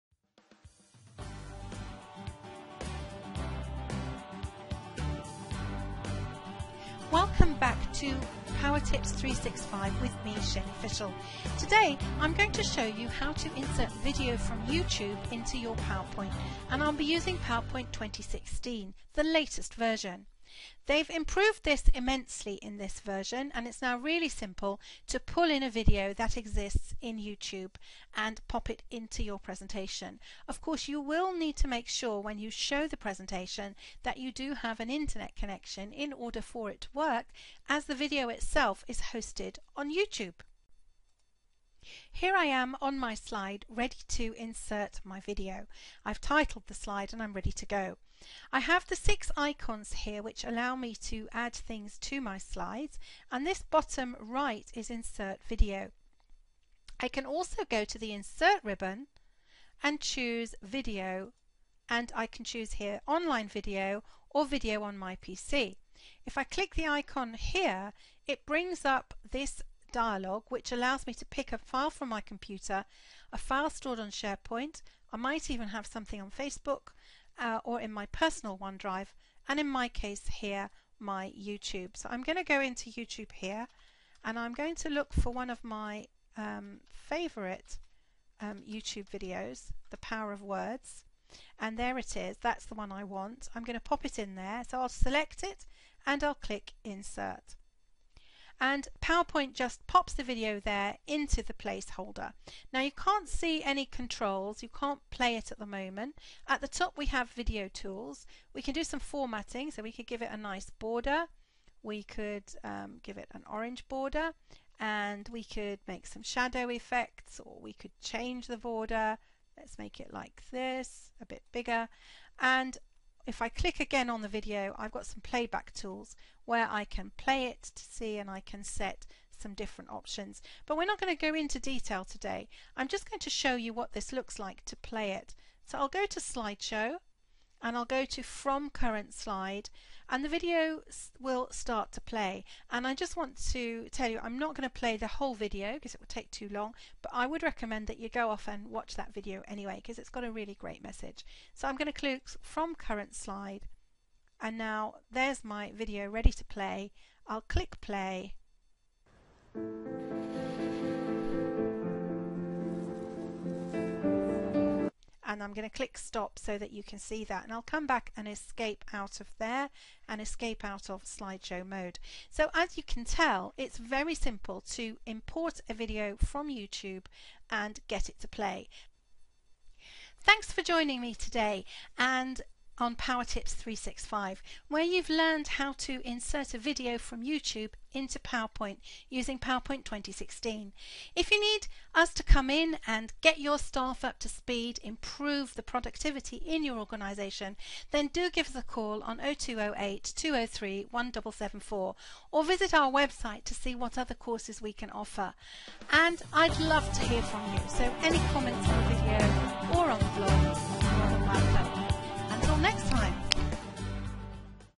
Insert Video from YouTube in PowerPoint 2016
Learn how easy it is to insert a video from YouTube into a PowerPoint 2016 presentation. Super simple.